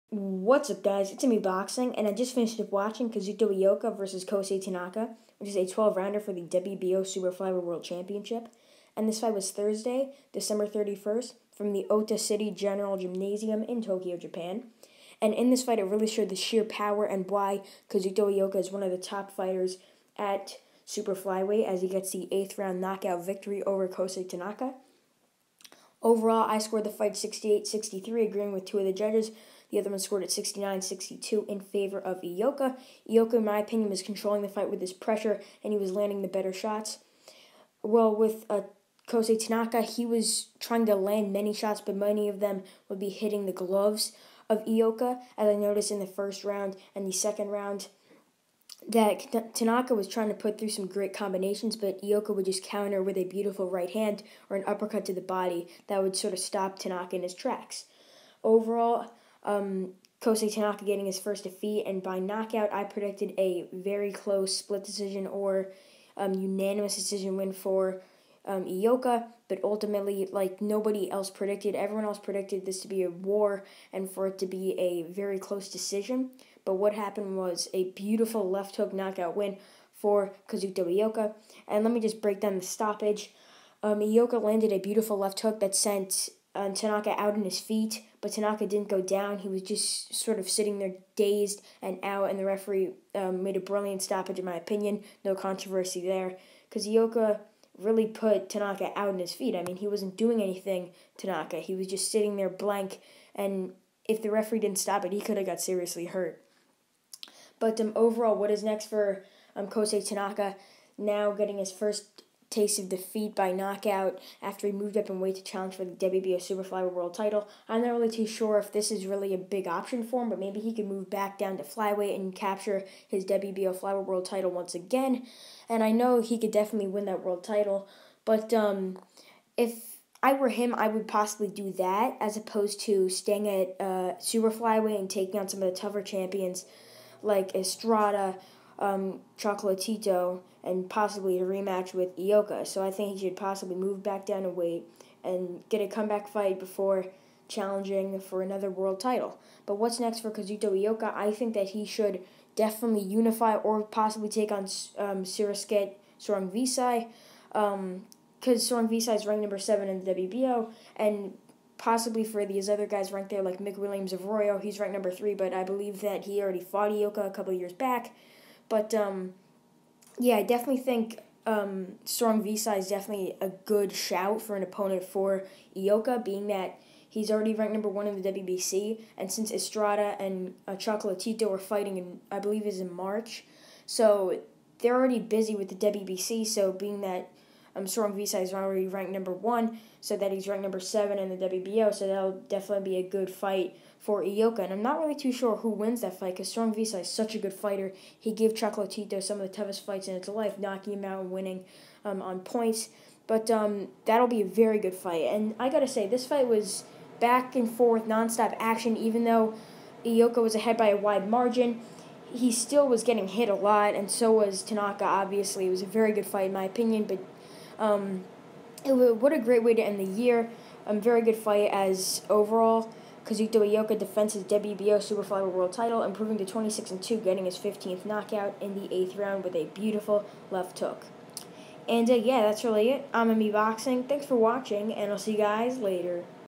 IOKA STOPS TANAKA!! Kazuto Ioka vs Kosei Tanaka Review/Result...WHATS NEXT?
On Thursday December 31st from the Ota-City General Gymnasium in Tokyo Japan, in a great fight, Kazuto Ioka stops Kosei Tanaka with a picture perfect left hook in the eighth round to retain his WBO super-flyweight world title.